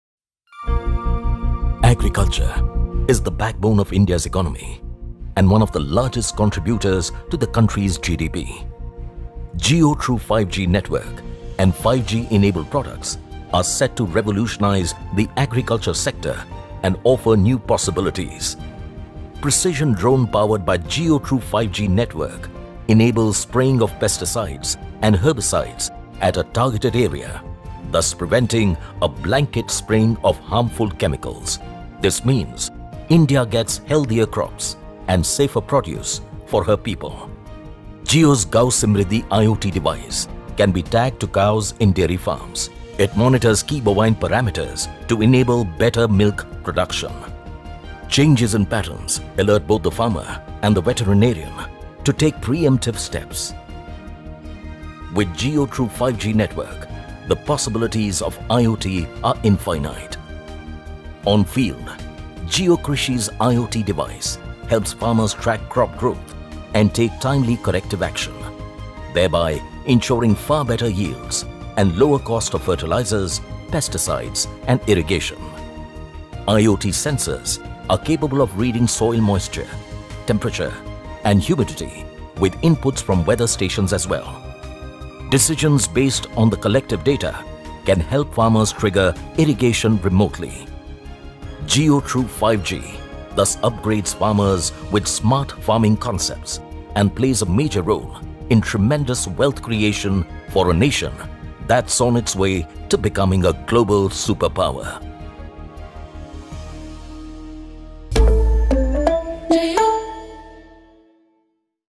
Bringing revolution in Agriculture sector with IoT-enabled devices powered by JioTrue5GIMC
Bringing revolution in Agriculture sector with IoT-enabled devices powered by JioTrue5G
India Mobile Congress

largest contributors to the country's GDP. JioTrue5G